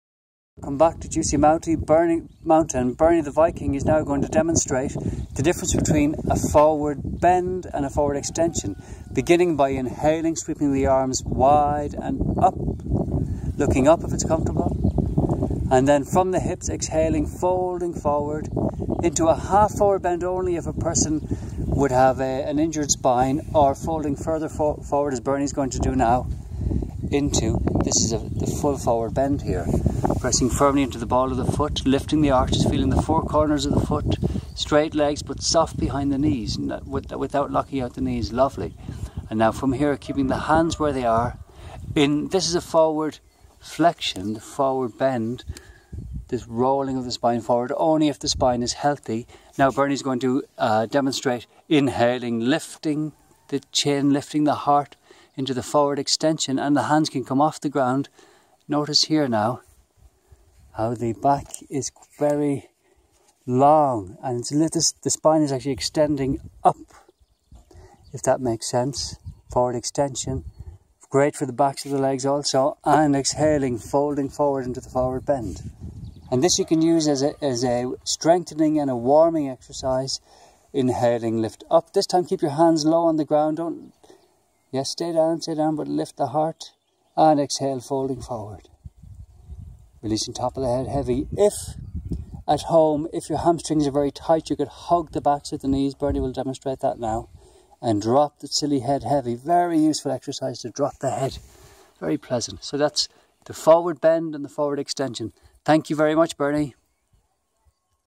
Standing Forward Bend and Extension
Bernie( the Viking ) demonstrates forward bending, the forward extension and the half forward bend for those who may have spinal difficulties or intervertebral disc compression. Thank you Bernie Hyde.